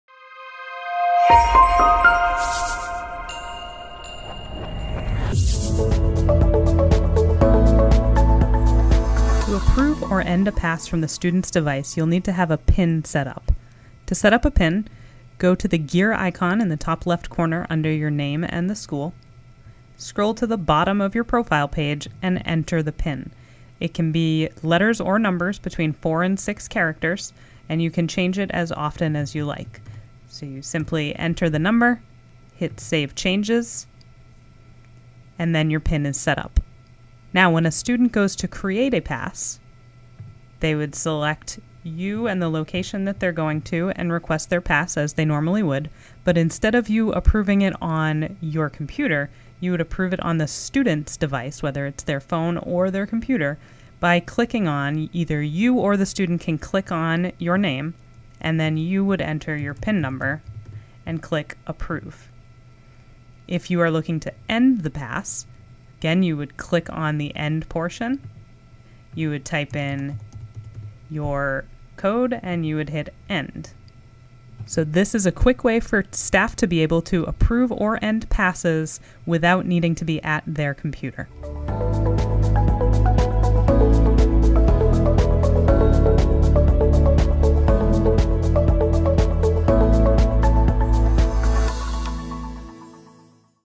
Using a PIN
How to use a PIN to approve passes from a student's device in Securly Pass